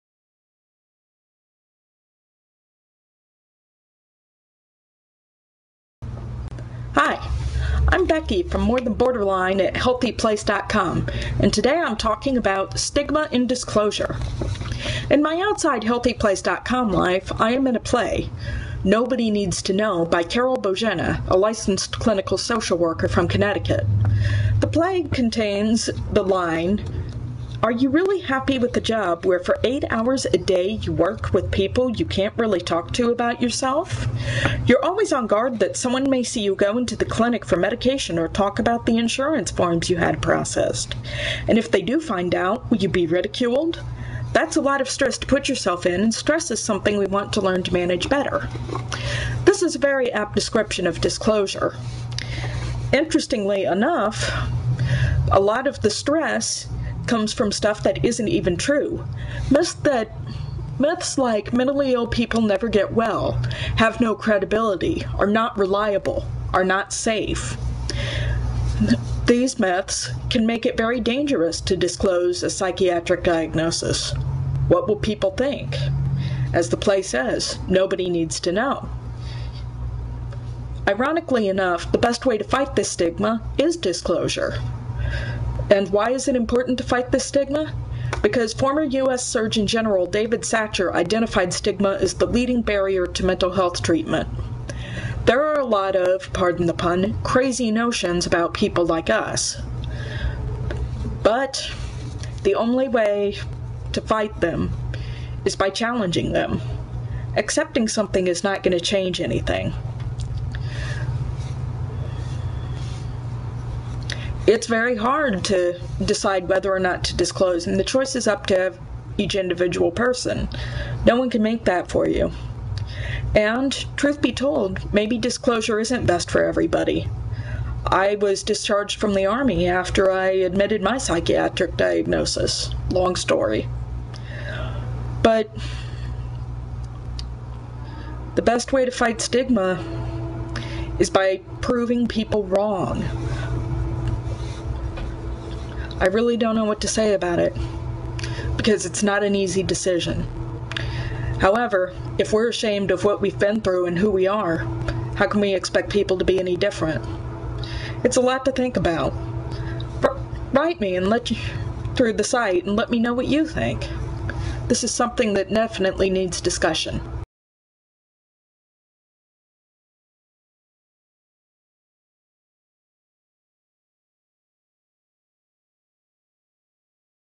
Borderline Personality Disorder: Disclosure As Anti-Stigma?
The stigma of mental illness is the leading barrier to its treatment, but is disclosure a way to fight it? More Than Borderline's Becky Oberg discusses living openly with a mental illness.